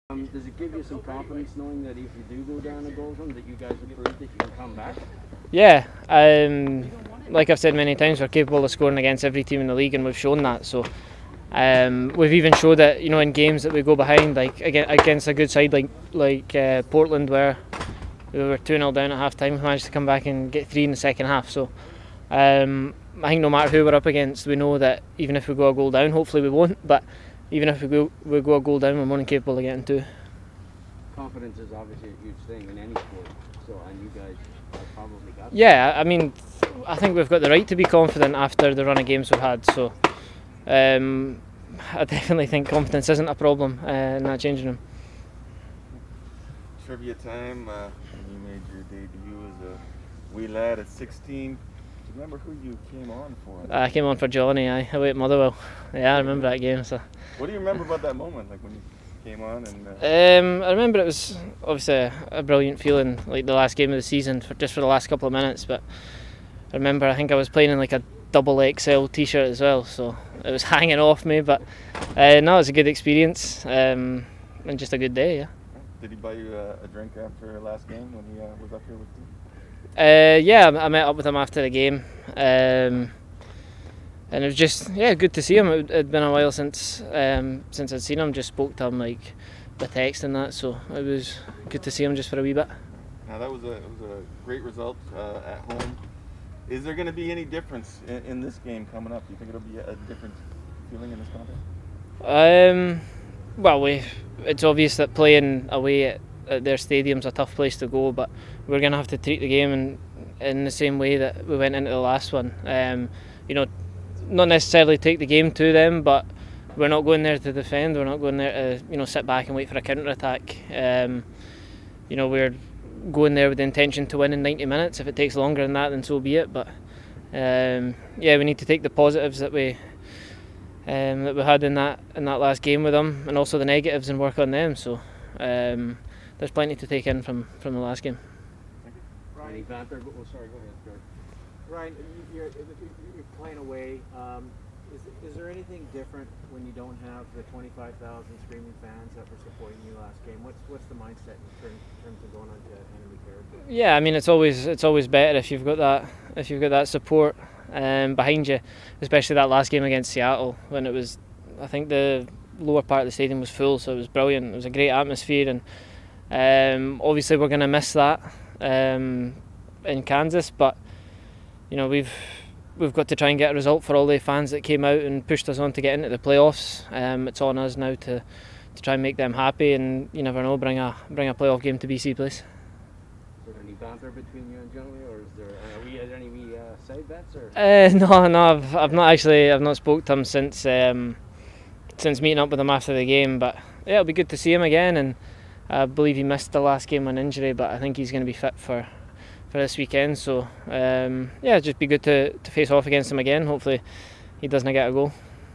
Media Availability: Ryan Gauld
Vancouver Whitecaps midfielder Ryan Gauld talks to the media ahead of the MLS playoff game against Sporting KC.